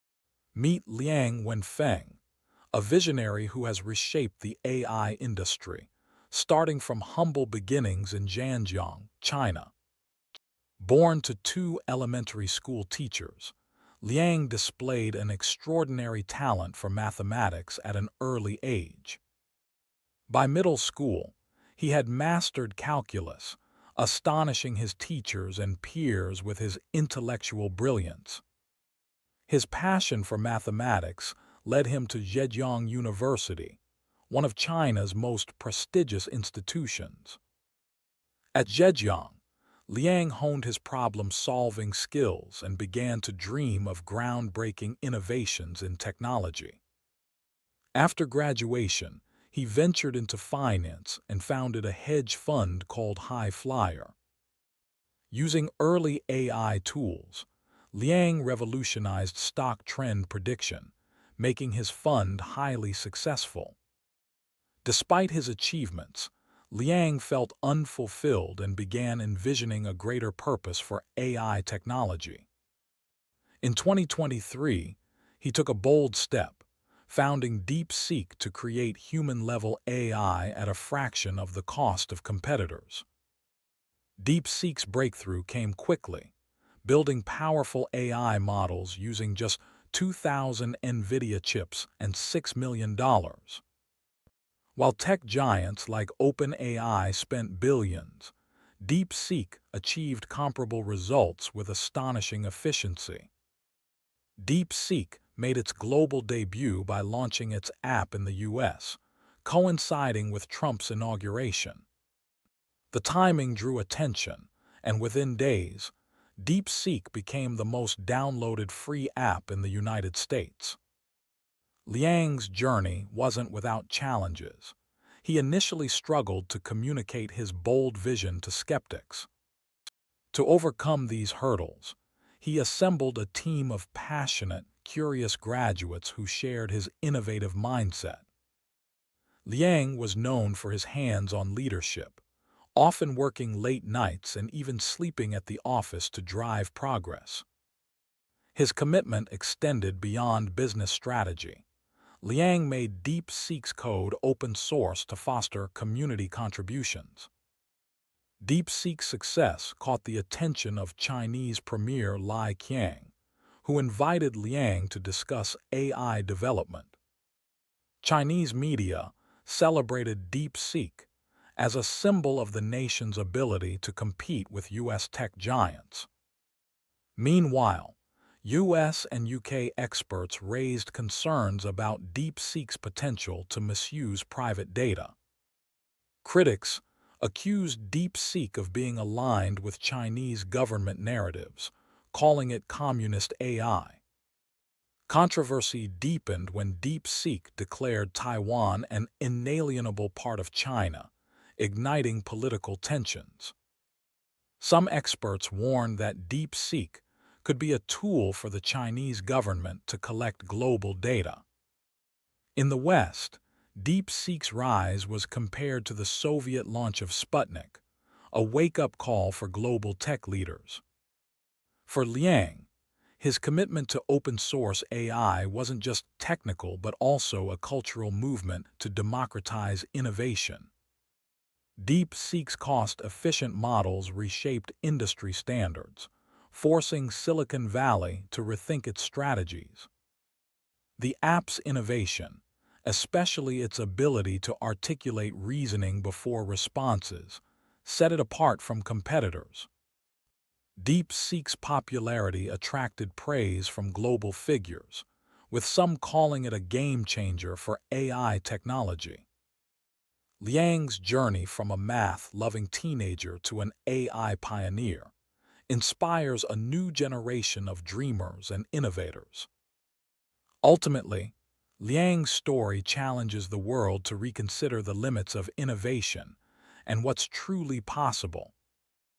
DeepSeek - From Unknown to AI Billionaire: How Liang Wenfeng’s DeepSeek is Shaking the World!
Description: "Liang Wenfeng went from a little-known entrepreneur to the mastermind behind DeepSeek—an AI company revolutionizing the tech world. With just $6 million and 2,000 Nvidia chips, DeepSeek has challenged the likes of billion-dollar giants like OpenAI and Silicon Valley. How did a math prodigy with a vision of human-level AI disrupt the entire industry? In this video, we dive deep into the astonishing rise of DeepSeek, its groundbreaking tech, and how it’s positioning China at the forefront of the global AI race. This is the story of one man’s bold dream to change the future—don’t miss it!"

Get ready to be inspired by the incredible journey of Liang Wenfeng, the visionary founder of DeepSeek, a groundbreaking AI company that’s taking the tech world by storm. In this video, we dive into his rise from humble beginnings in Zhanjiang to becoming one of the most influential figures in the AI revolution. Liang’s innovative approach to artificial intelligence has disrupted the industry, challenging tech giants and proving that you don’t need billions of dollars to lead in the AI space.

From his early love for mathematics to building a successful hedge fund, and later founding DeepSeek in 2023, Liang has reshaped the future of AI. With his open-source philosophy and a team of passionate graduates, DeepSeek is revolutionizing AI efficiency, producing human-level AI at a fraction of the cost of its competitors. The app quickly became one of the most downloaded in the US, and its impact continues to grow globally.

But with success comes controversy. From geopolitical tensions to concerns about data privacy, DeepSeek’s rise is sparking debates across the world. Is it the next big thing in AI, or is it too much for the world to handle?

Join us as we explore Liang Wenfeng's journey, his vision for AI, the rise of DeepSeek, and the impact this revolution is having on the tech world and beyond. Watch till the end to see how DeepSeek is not only changing AI but also challenging the balance of global tech power. If you’re a fan of innovation, tech breakthroughs, or just want to know what’s next in the AI world, this video is a must-watch!